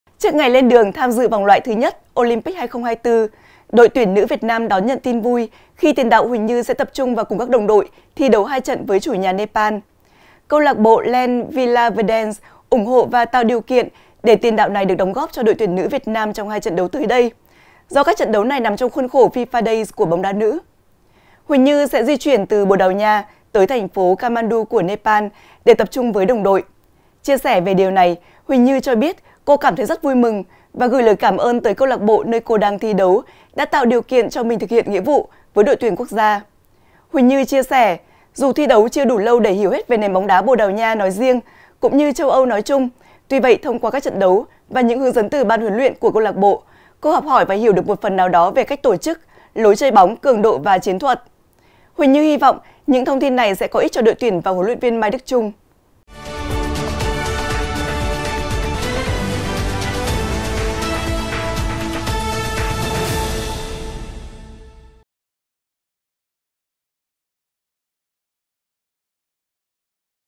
Huỳnh Như báo tin vui trước vòng loại Olympic Paris
Trước ngày lên đường tham dự vòng loại thứ nhất Olympic 2024, ĐT nữ Việt Nam đón nhận tin vui khi tiền đạo Huỳnh Như sẽ tập trung và cùng các đồng đội thi đấu 2 trận với chủ nhà Nepal.
CLB Lank Vilaverdense ủng hộ và tạo điều kiện để tiền đạo này được đóng góp cho ĐT nữ Việt Nam trong 2 trận đấu tới đây do các trận đấu này nằm trong khuôn khổ FIFA Days của bóng đá nữ. Huỳnh Như sẽ di chuyển từ Bồ Đào Nha tới thành phố Kathmandu (Nepal) để tập trung với đồng đội.
Chia sẻ về điều này, Huỳnh Như cho biết: “Như cảm thấy rất vui mừng và gửi lời cám ơn tới CLB Lank Vilaverdense tạo điều kiện trở về thực hiện nghĩa vụ cho ĐTQG. Như rất tự hào mỗi lần được khoác lên chiếc áo của ĐT nữ Việt Nam và luôn cống hiến hết mình. Như cùng toàn đội quyết tâm cao đạt kết quả tốt nhất trong các trận đấu thuộc vòng loại Olympic lần này.

37 Bà Triệu, Hoàn Kiếm, Hà Nội, Việt Nam